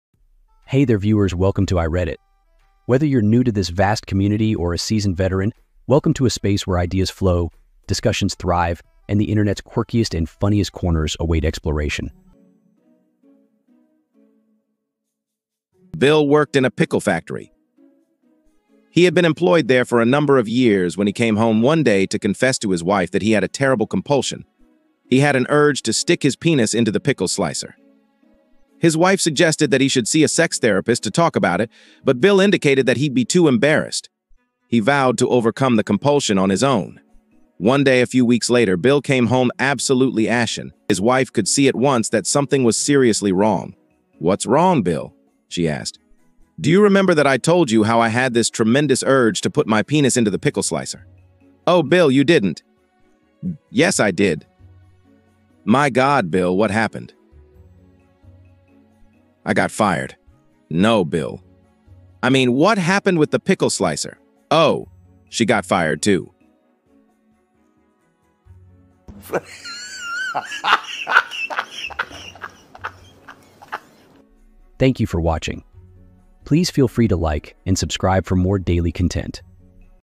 😂Today's Best Joke😂 - Bill Just Needed Some Help
We will be sending content your way everyday and we hope to build a safe community which provides laughter, counsel and comfort!

                                                              👇Joke 👇
Bill worked in a pickle factory. He had been employed there for a number of years when he came home one day to confess to his wife that he had a terrible compulsion. He had an urge to stick his penis into the pickle slicer. His wife suggested that he should see a sex therapist to talk about it, but Bill indicated that he'd be too embarrassed.

He vowed to overcome the compulsion on his own. One day a few weeks later, Bill came home absolutely ashen. His wife could see at once that something was seriously wrong. "What's wrong, Bill?" she asked. "Do you remember that I told you how I had this tremendous urge to put my penis into the pickle slicer?" "Oh, Bill, you didn't." "Yes, I did." "My God, Bill, what happened?" "I got fired." "No, Bill. I mean, what happened with the pickle slicer?" "Oh...she got fired too."